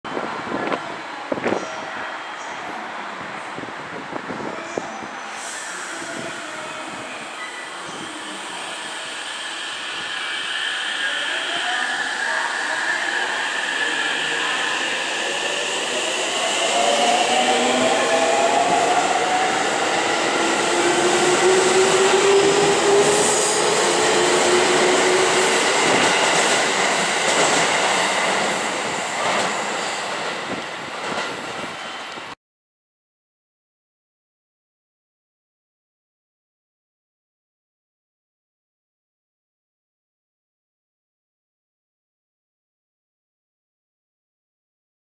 令和5年3月13日！J162編成+J153編成新快速豊橋行き 駅列車特集 JR東海道本線 名古屋駅2番線 その308
本日は3月13日なので313系の日ですね。90編成が所属している大垣車両区の313系。今では3+3の新快速も見れるようになりましたが2年前までは見れなかったですね。